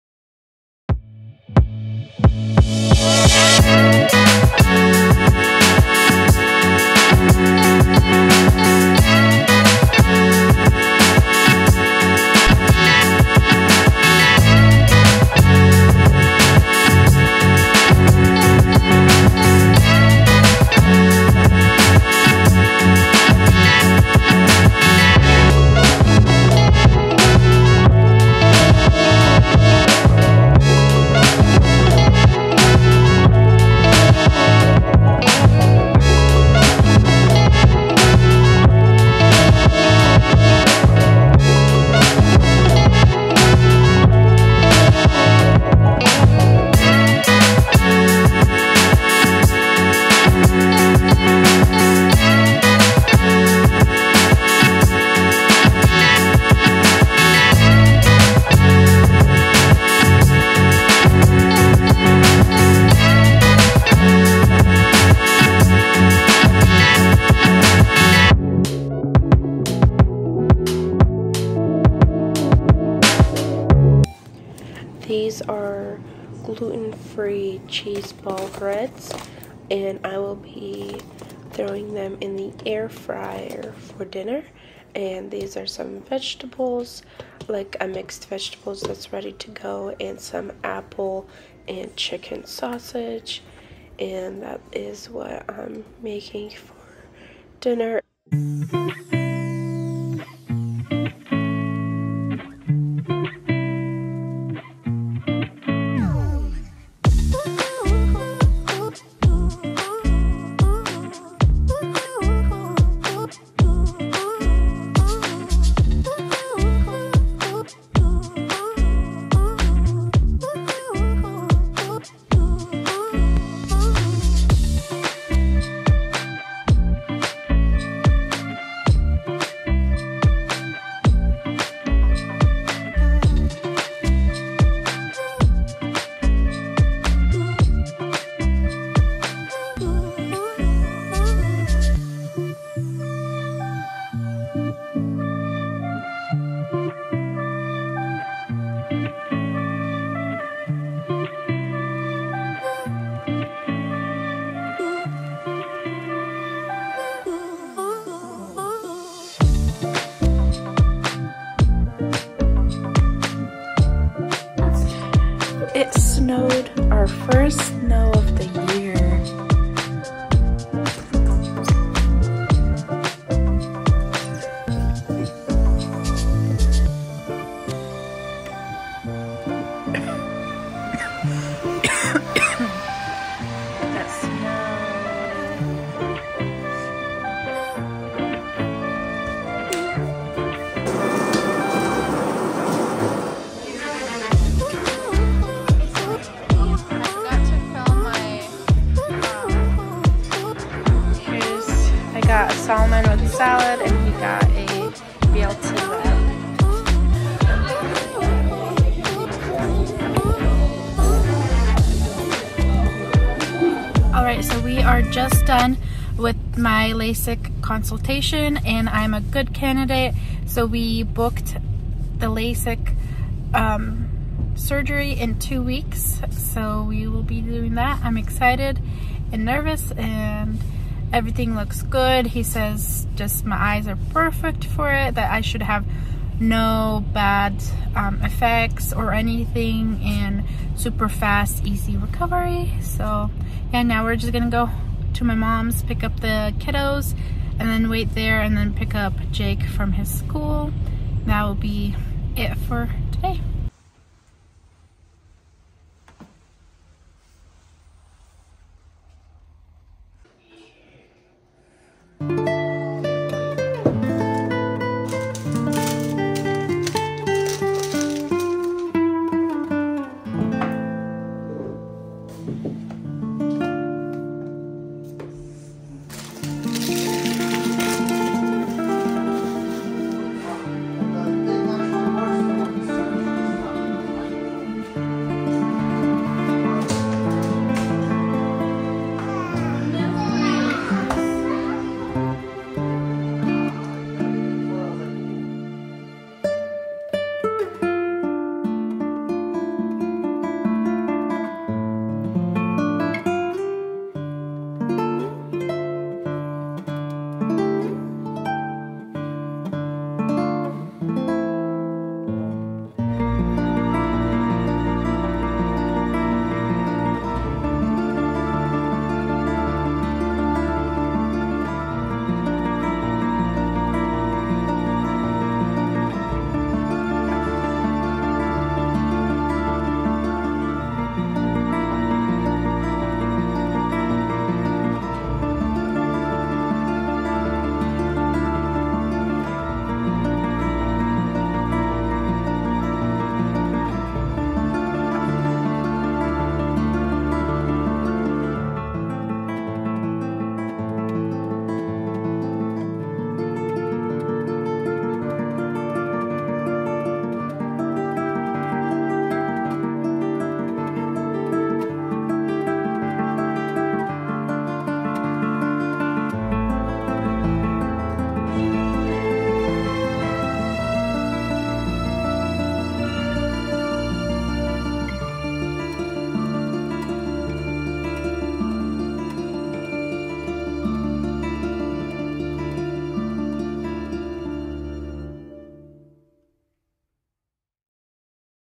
Lasik Consultation Appointment
Hey World today Ive a long decided decision has been made. Im having an lasik consultation. Hope you all enjoy this video and have a blessed day. Love Fig Fam.